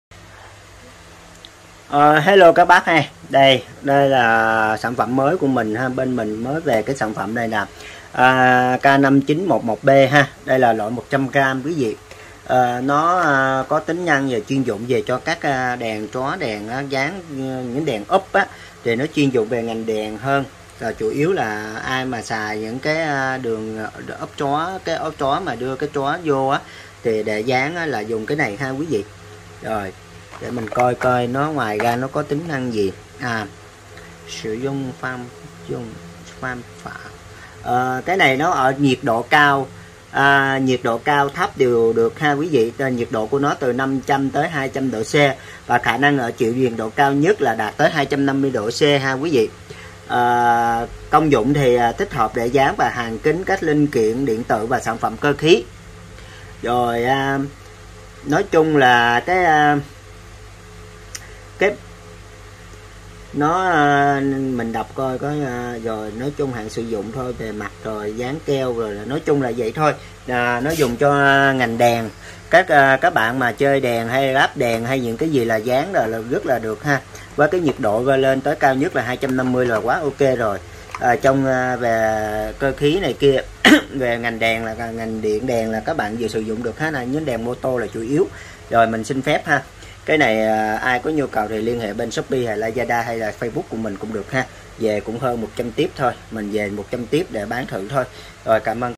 Keo Đen Kafuter K - 5911 B Chuyên Dụng Độ Bi Led - Xenon.
Đen Kafuter K - 5911B Chuyên Dụng Độ Bi Led - Xenon.
✨️Keo dạng típ là sản phẩm chuyên dụng ngành đèn
  Sản phẩm để ae vào bi vào choá để gắn. Khe hở gắn chụp xi
  Sản phẩm tương tự keo silicon chịu nhiệt độ cao 250 °C và không có axit như keo silicon nên không bị ăn mòn hay oxi hoá.
  Thơi gian khô bề mắt 10-15 phút
  Thời gian khô hoàn toàn 1-2 tiếng
  Dùng làm zăng chống nước cho bi gầm và những chỗ cần làm kín và giữ cố định
  Khi khô hoàn toàn keo có độ bám dính cao cứng chắc
✨️✨️Công dụng: Thích hợp để dán và hàn kín các linh kiện điện tử và sản phẩm cơ khí.